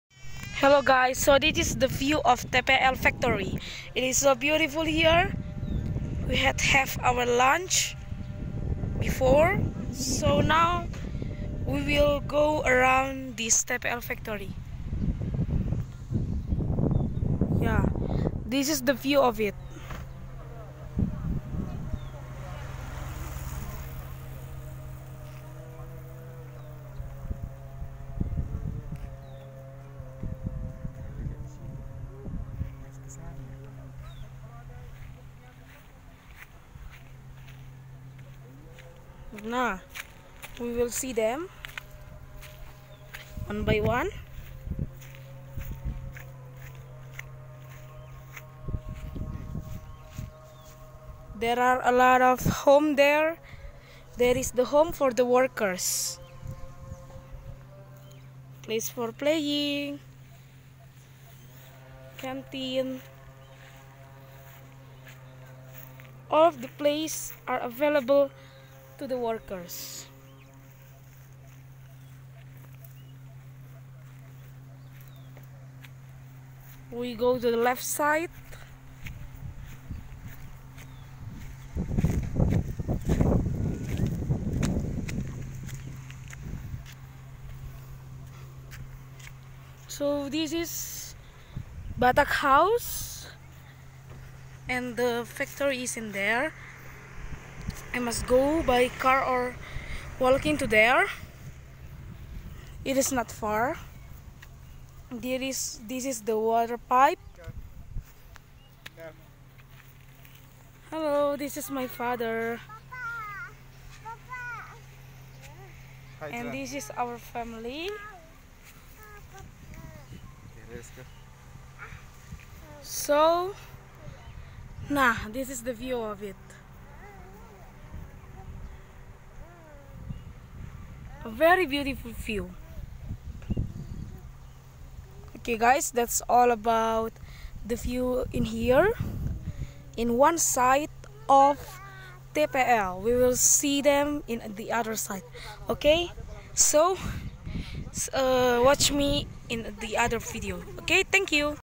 Toba Pulp Lestari 3
Jalan jalan melihat lapangan tenis dan lapangan sepak bola, sport area di Toba Pulp Lestari.